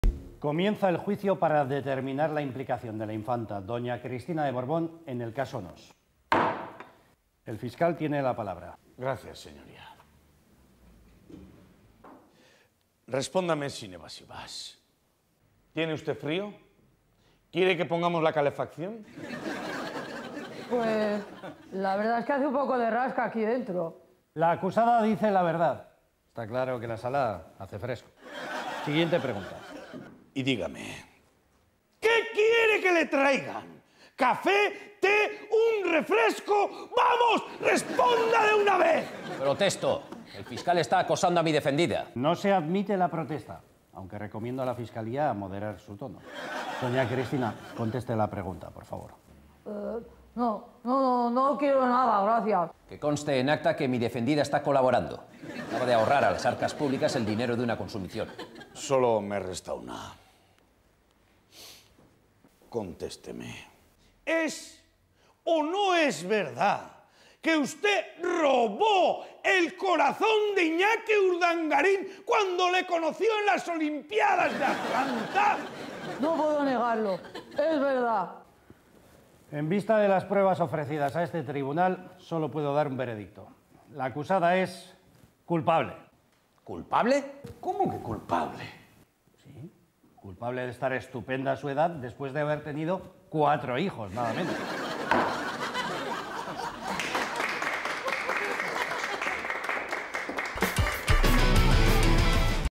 La infanta acusada de robar el corazón de Urdangarin
En pleno juicio del caso Noos, la Infanta Cristina de Borbón ha sido acusada de robar. El juez ha dictaminado que robó el corazón de Iñaki Urdangarin.